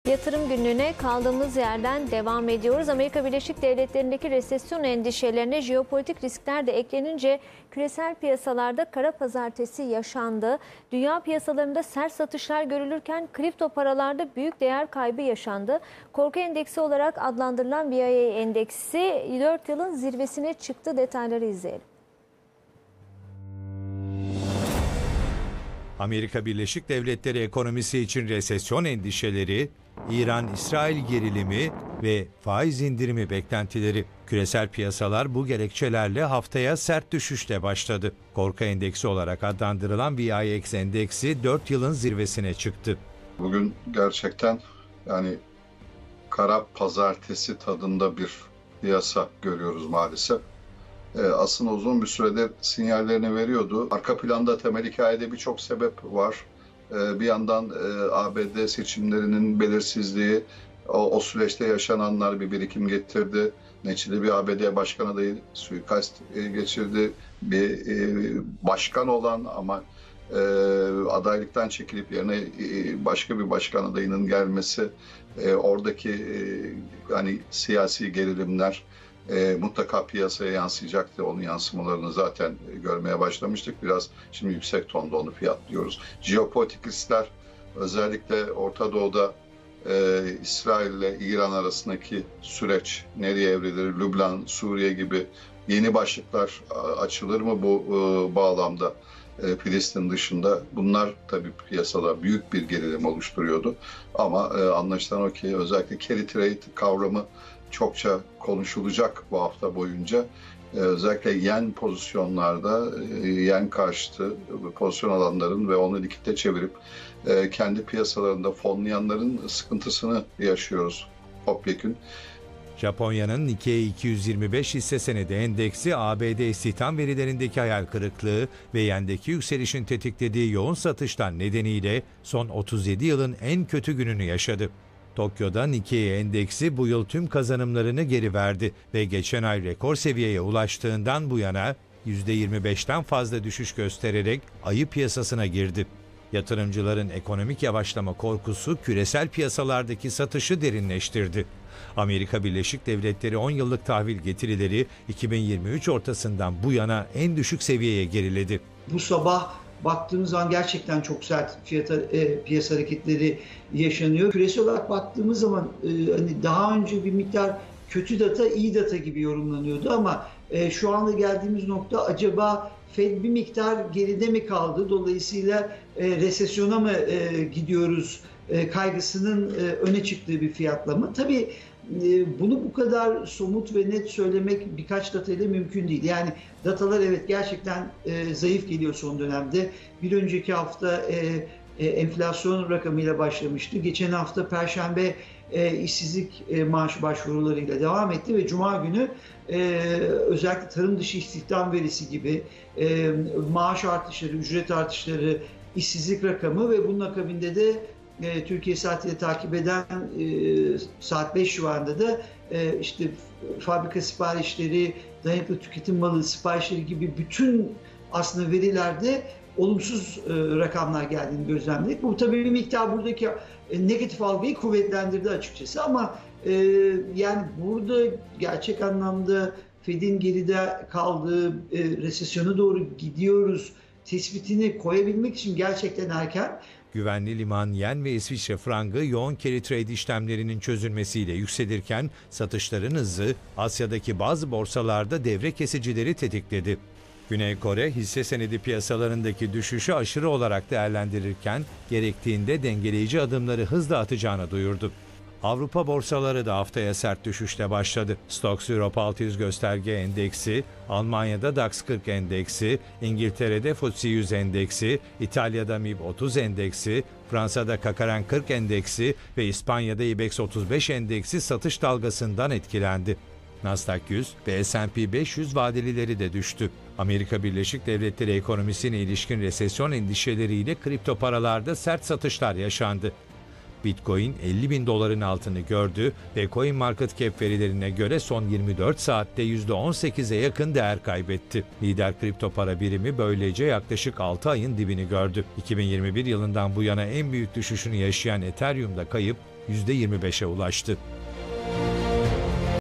Küresel Piyasalarda "Kara Pazartesi" l A Para
Küresel piyasalarda deyim yerindeyse "kara pazartesi" yaşıyor. ABD'nin resesyona gireceği korkusu artarken, Ortadoğu'da gerilim had safhaya ulaştı. Riskli varlıklarda satışlar ABD'den Asya'ya dalga dalga yayıldı.